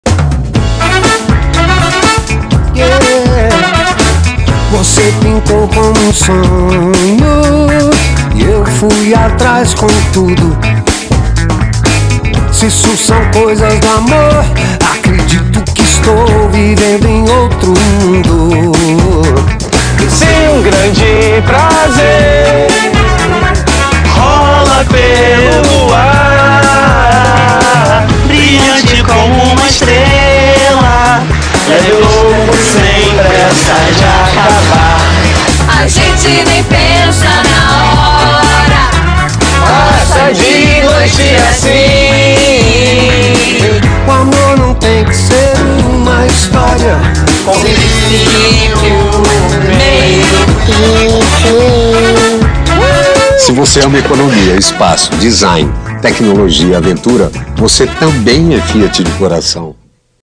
Fiat Fabio Jr Sonho 60.mov
Propaganda Fiat Fabio Jr. - Sandro Haick Produção Musical